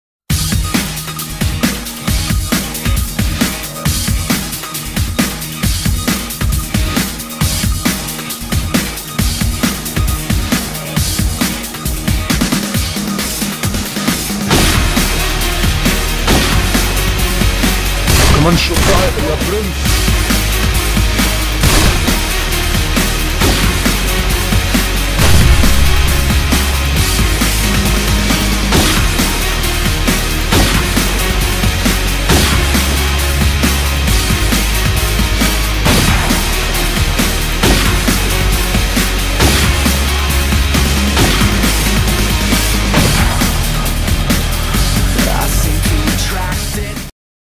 PAIN // ente
some random frags from cw//tcw also in random servers for fun :)